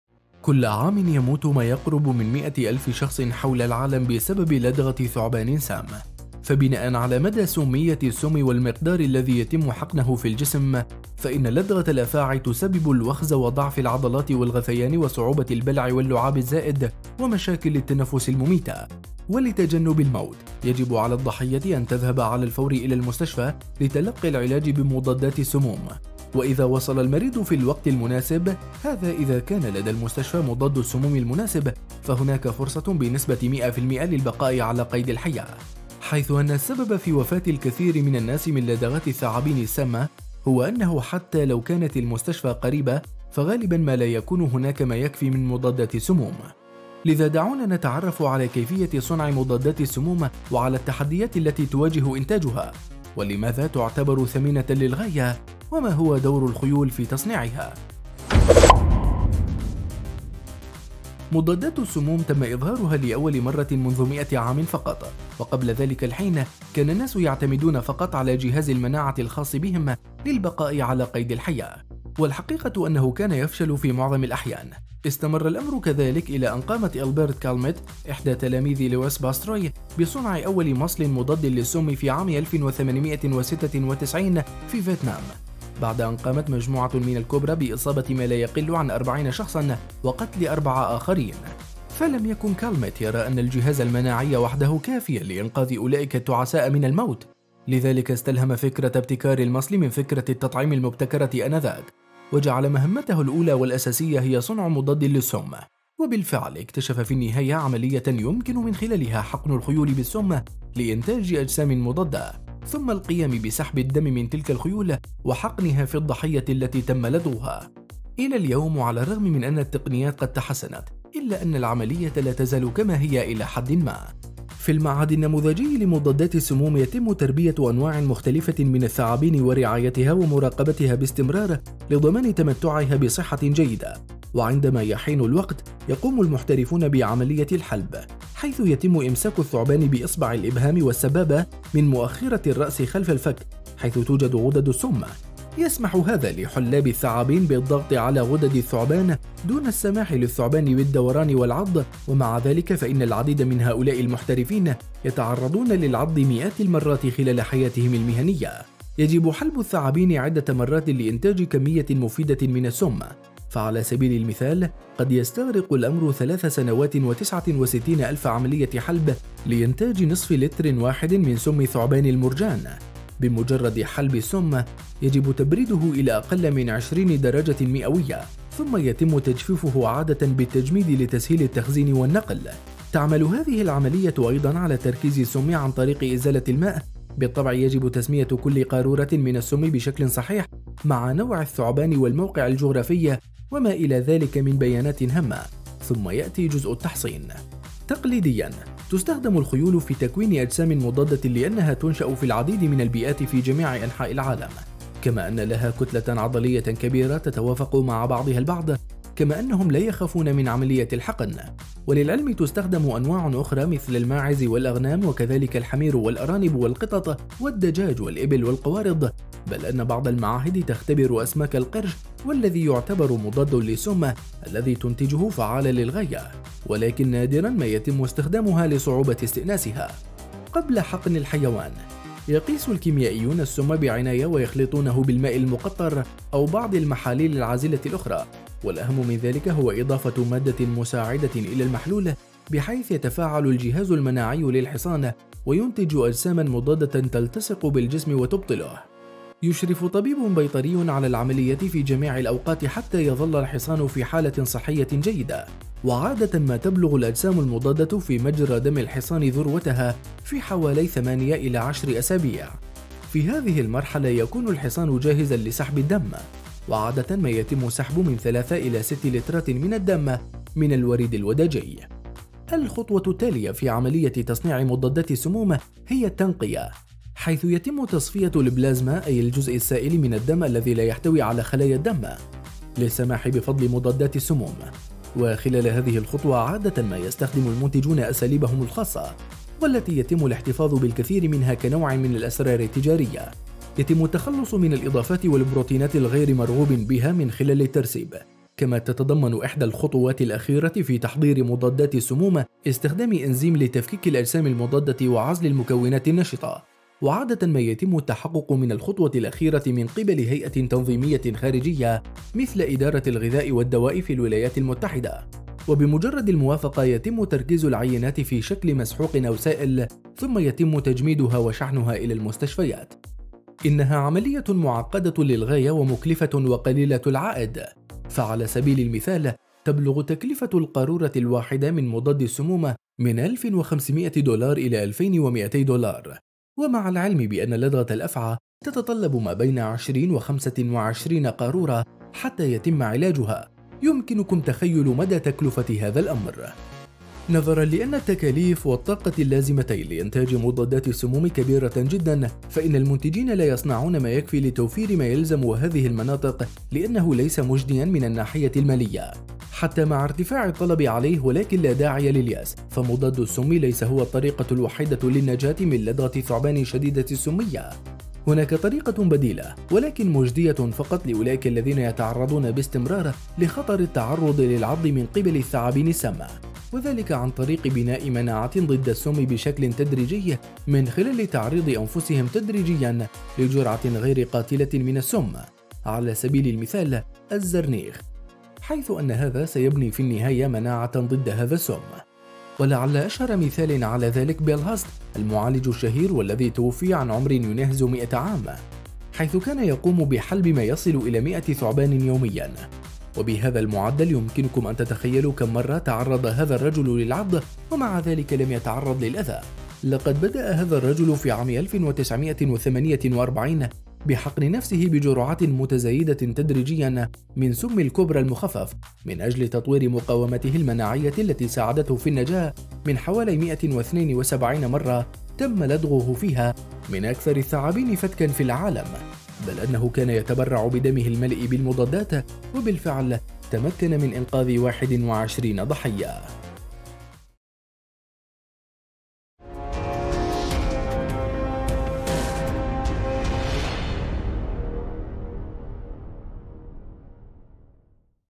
كيف تنقذ الخيول البشر من لدغات الثعابين ؟
كل عام  يموت ما يقرب من 100 الف شخص حول العالم بسبب لدغة ثعبان سامة . فبناءً على مدى سمية السم و المقدار الذي يتم حقنه في الجسم ، فإن لدغة الأفاعي تسبب الوخز وضعف العضلات والغثيان وصعوبة البلع و اللعاب الزائد و مشاكل التنفس المميتة .
و لتجنب الموت ، يجب على الضحية أن تذهب على الفور إلى المستشفى لتلقي العلاج بمضادات السموم . و إذا وصل المريض في الوقت المناسب – هذا اذا كان لدى المستشفى مضاد السموم المناسب - فهناك فرصة بنسبة 100٪ للبقاء على قيد الحياة . حيث أن السبب في وفاة الكثير من الناس من لدغات الثعابين السامة هو أنه حتى لو كانت المستشفى قريبة ، فغالبًا ما لا يكون هناك ما يكفي من مضادات السموم

الان يمكنك الحصول على كورس الفان جروب "أسرار عالم اليوتيوب" بخصم 25% , انطلق الان ونافس اكبر صناع المحتوى العرب